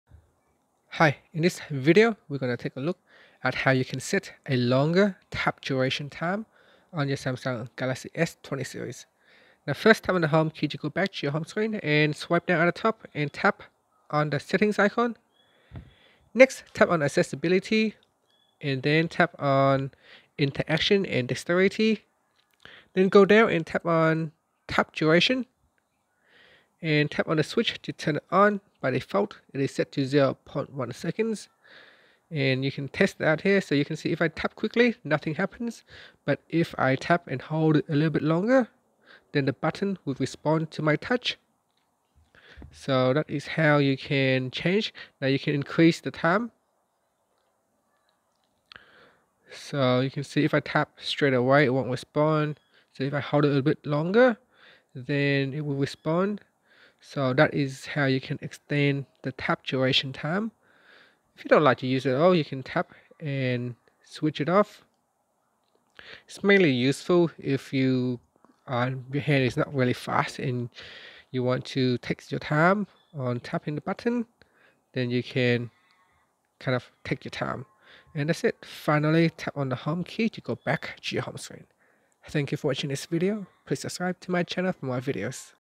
Galaxy S20/S20+: How to Set a Longer Tap Duration Time
Learn how you can set a longer tap duration time on Galaxy S20 / S20 Plus / S20 Ultra.

Android 10.